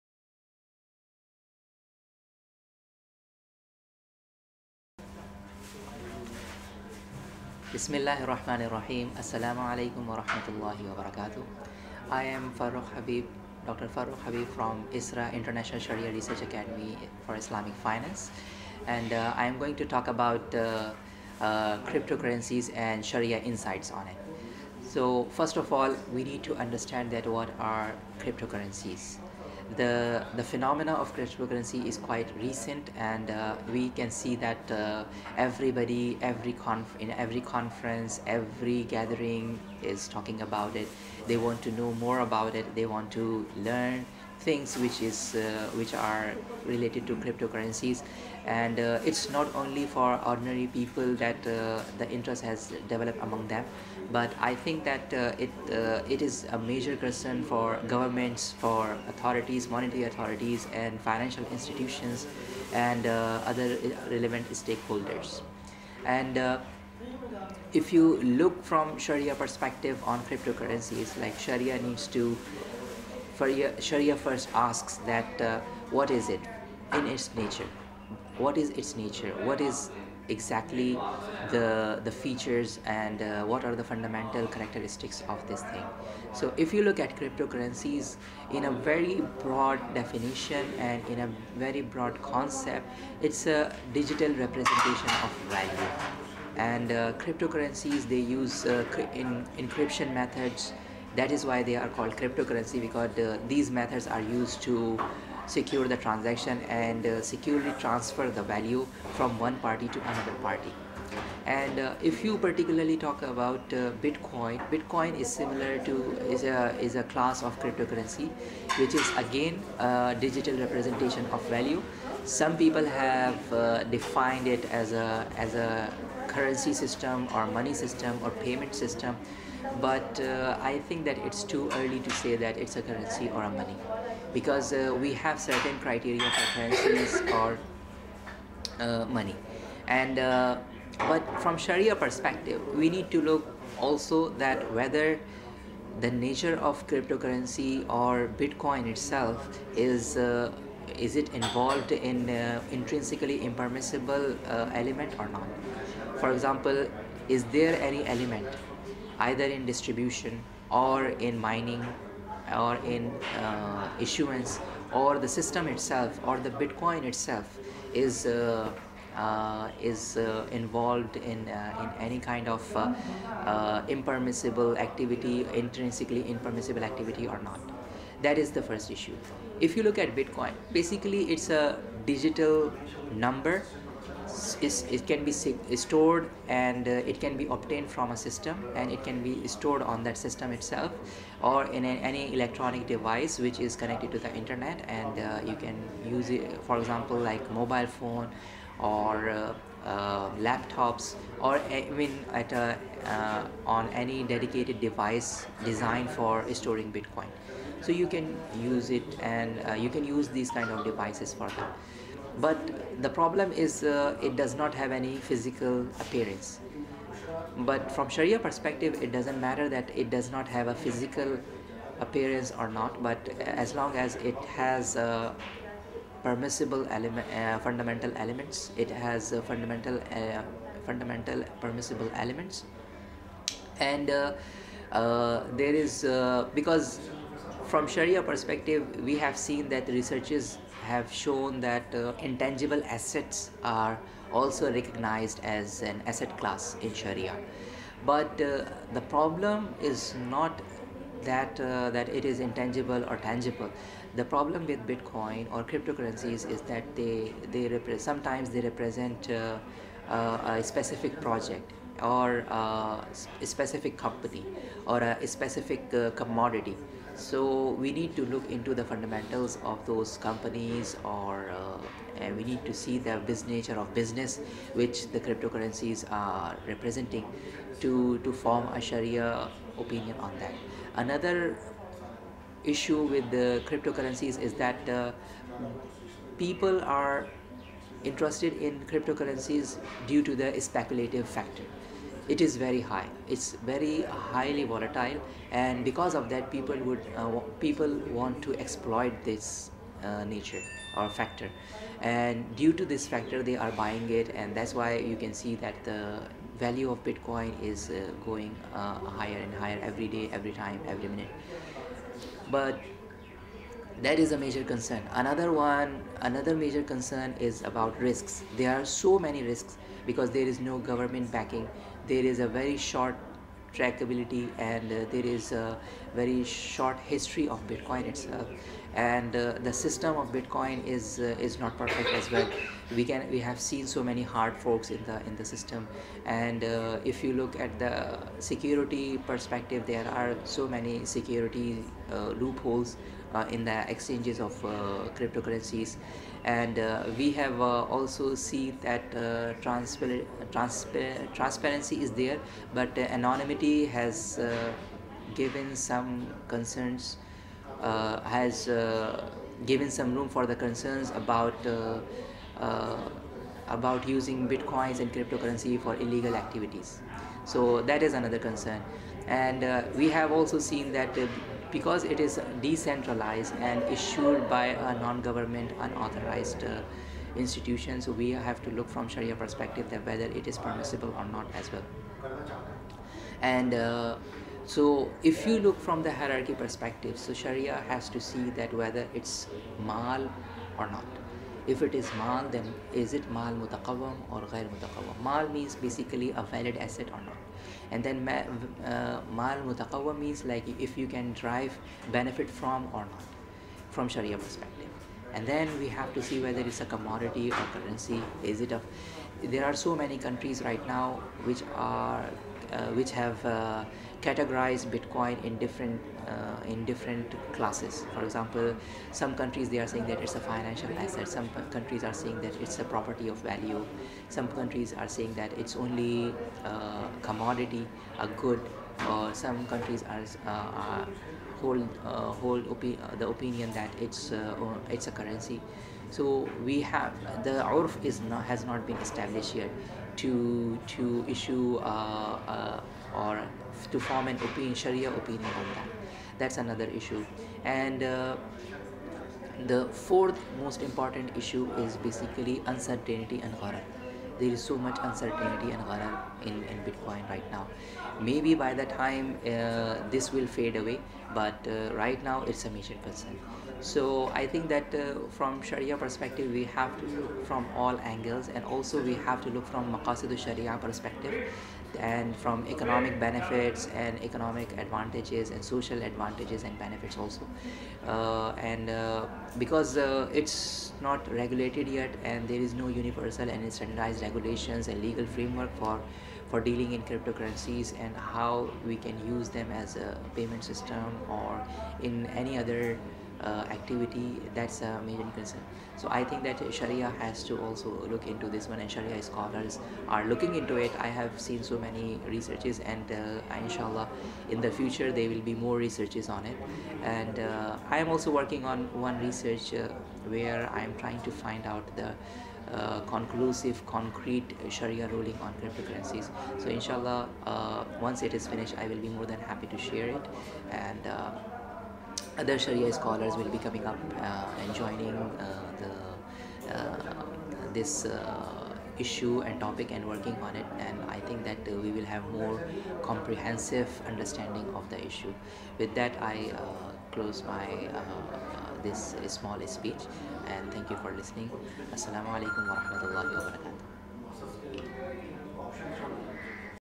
CEIF Talks- "Shariah Perspective on Cryptocurrencies" by Dr. Farrukh Habib
Dr. Farrukh Habib, Researcher, ISRA, INCEIF gives a shariah perspective on cryptocurrencies and the factors affecting its permissibility in Islam.